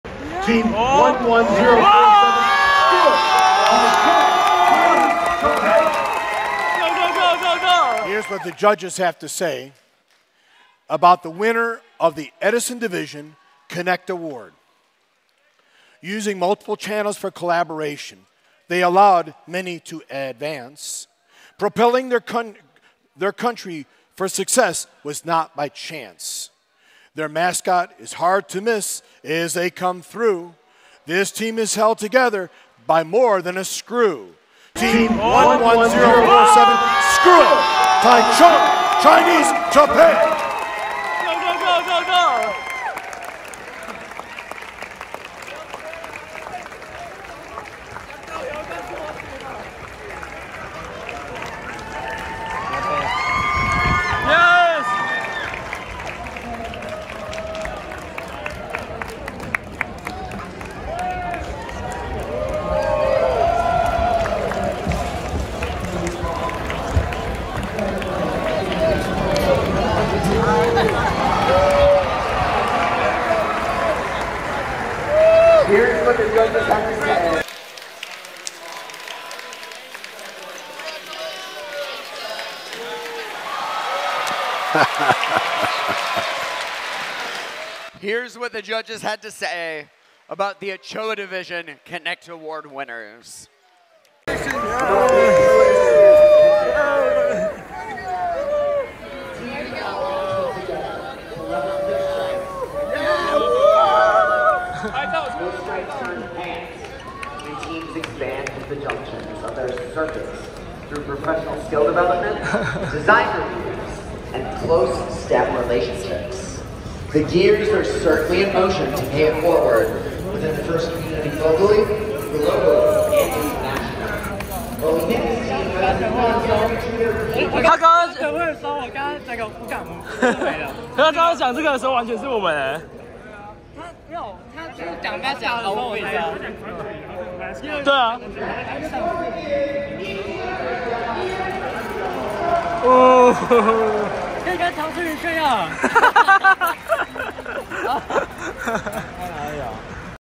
Connect Award🏆- 2023 FIRST World Championship in Houston FTC Power Play | FTC Team 11047 Screw it !!
【FTC 台灣代表隊 11047】榮獲世界賽獎項 - 最佳團隊組織獎(CONNECT AWARD WINNER) !!!
感謝今年的贊助商：MATRIX、陽光城市、九天民俗技藝團

Connect Award(最佳團隊組織獎)：
該獎項授予與當地社區和工程界聯繫最緊密的團隊。一個真正的 FIRST 團隊不僅僅是其各個部分的總和，並且認識到其學校和社區對他們的成功起著至關重要的作用。該獎項的獲得者因幫助社區了解 FIRST、FTC 和團隊本身而受到認可。贏得該獎項的團隊正在積極尋找工程師並探索工程、科學和技術領域的可用機會。此外，該團隊有清晰的業務或戰略計劃，並確定了實現目標的步驟。

獲獎指南：
1.團隊向所有人展示尊重和親切的專業精神
必須提交工程筆記本，並且必須包括業務或戰略計劃。
2.團隊提供了明確的社區外展示例
3.團隊致力於與工程、科學或技術社區建立面對面或虛擬的聯繫
4.團隊有一個業務或戰略計劃，確定他們未來的目標以及他們為實現這些目標將採取的步驟。該計劃可以包括籌款目標、可持續性目標、時間表、外展和社區服務目標。

【FTC Taiwan Team 11047】Won the World Championship Award - CONNECT AWARD WINNER !!!
Thanks to this year's sponsors：MATRIX、Taichung City Sunshine City Development Association、CHIO-TIAN FOLK DRUMS & ARTS TROUPE